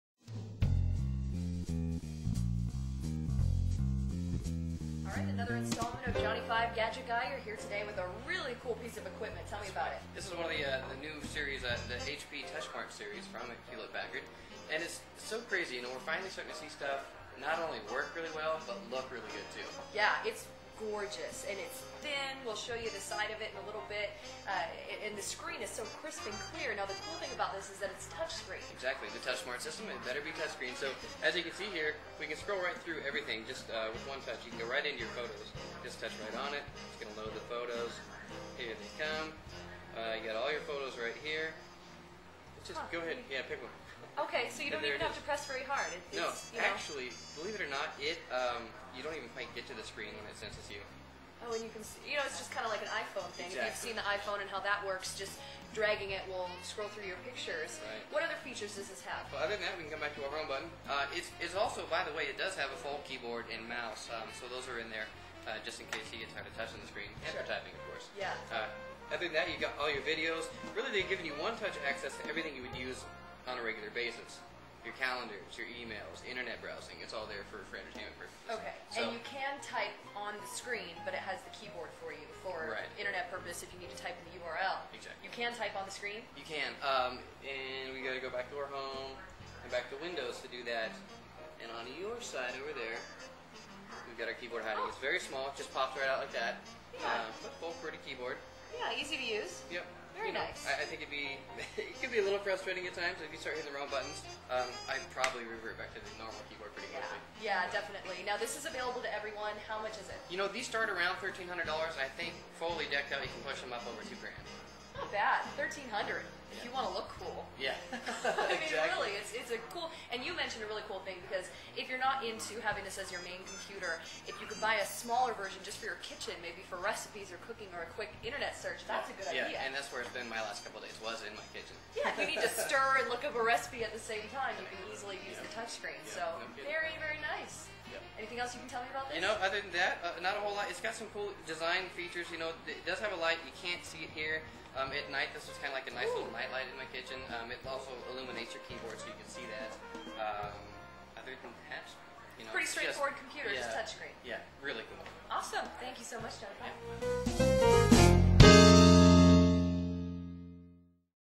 The HP Touchsmart PC
Gadget Guy Johnny 5 shows off his latest gadget - the HP Touchscreen PC to KXLY4's Colleen O'Brien/